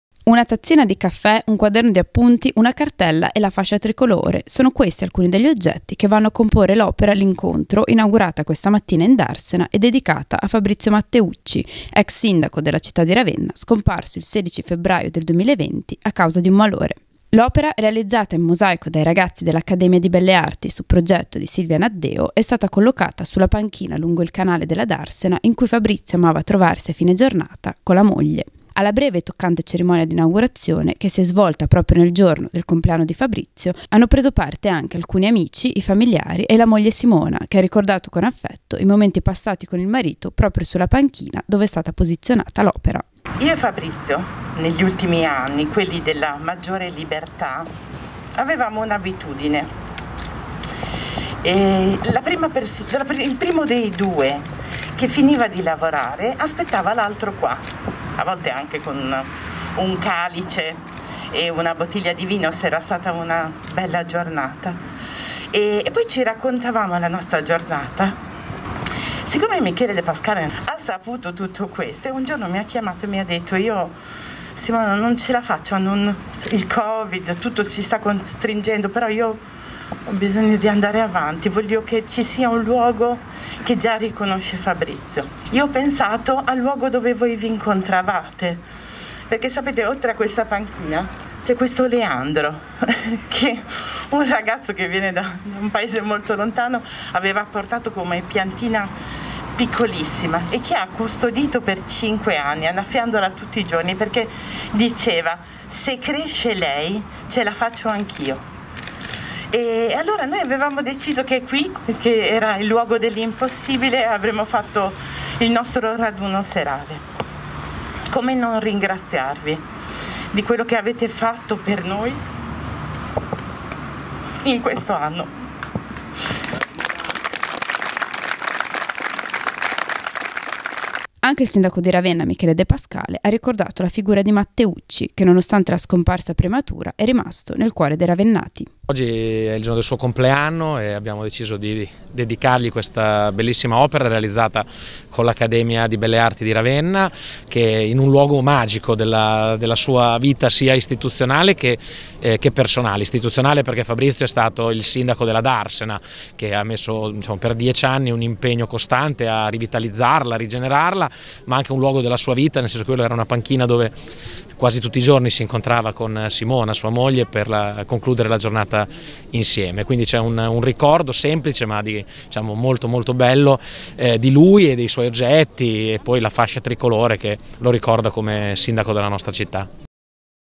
Nel giorno del suo compleanno, inaugura in Darsena l'opera in mosaico dedicata a Fabrizio Matteucci
Una tazzina di caffè, un quaderno di appunti, una cartella e la fascia tricolore: sono questi alcuni degli oggetti che vanno a comporre l'opera "L'incontro" inaugurata questa mattina in Darsena e dedicata a Fabrizio Matteucci, ex sindaco della città di Ravenna scomparso il 16 febbraio del 2020 a causa di un malore. L'opera, realizzata in mosaico dai ragazzi dell'Accademia di Belle Arti su progetto di Silvia Naddeo, è stata collocata sulla panchina lungo il canale della Darsena in cui Fabrizio amava trovarsi a fine giornata con la moglie. Alla breve e toccante cerimonia d'inaugurazione, che si è svolta proprio nel giorno del compleanno di Fabrizio, hanno preso parte anche alcuni gli amici, i familiari e la moglie Simona, che ha ricordato con affetto i momenti passati con il marito proprio sulla panchina dove è stata posizionata l'opera. Anche il sindaco di Ravenna Michele de Pascale ha ricordato la figura di Matteucci, che nonostante la scomparsa prematura è rimasto nel cuore dei ravennati.